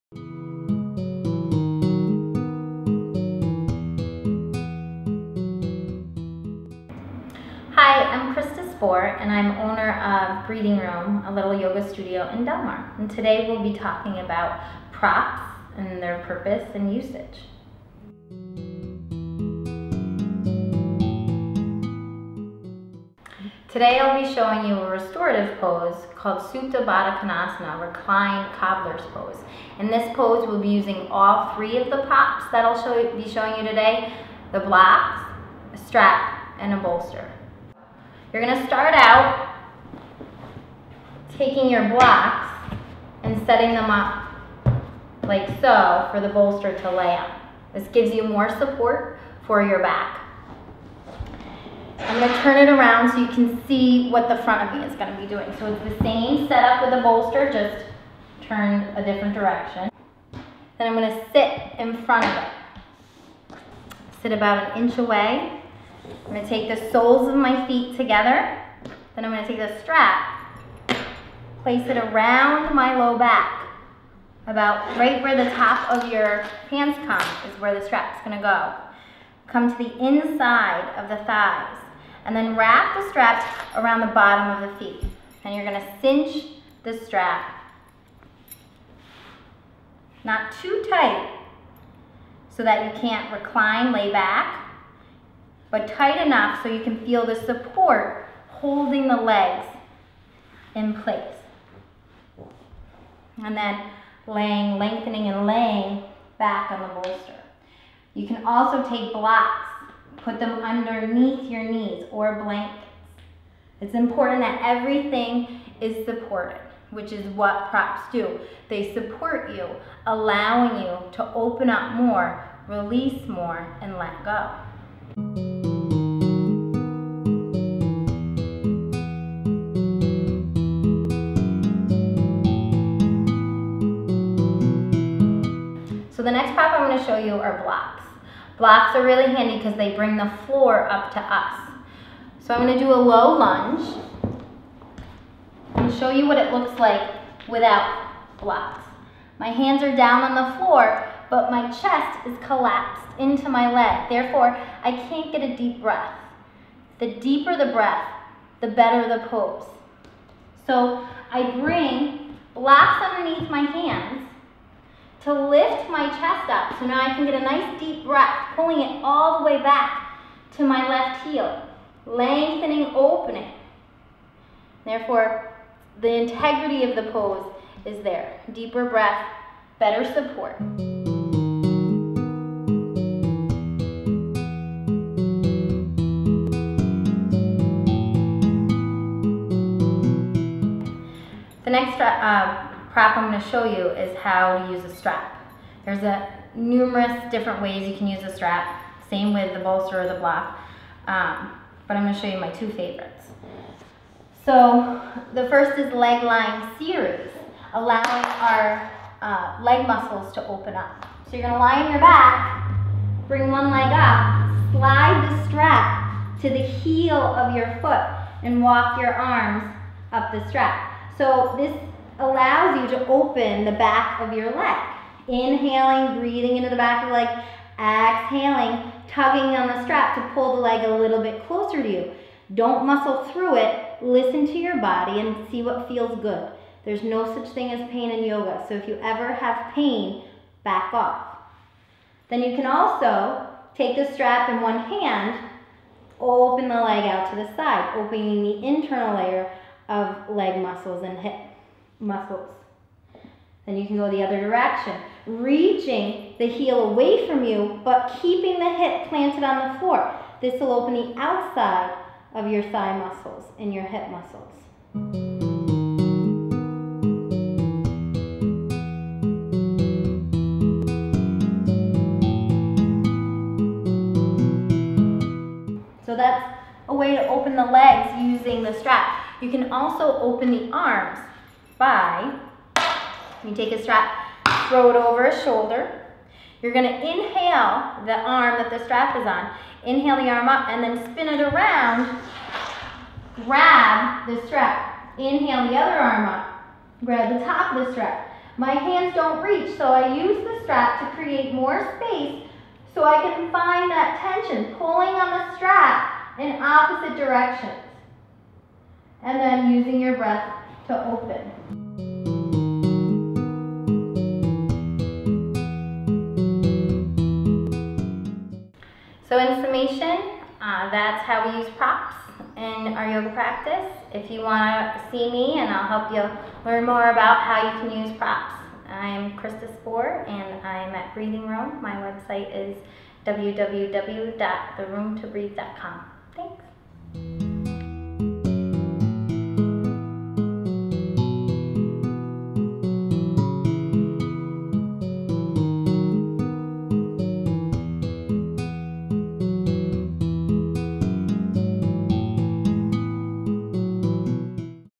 Using Props to Assist in Your Yoga Pose
518Life magazine presents "Yoga Props: Using blocks, bolsters and straps to assist in your yoga poses". Instructed by Krista Spohr, owner and yoga instructor of Breathing Room in Delmar, NY.

Video by Colleen Ingerto/518Life.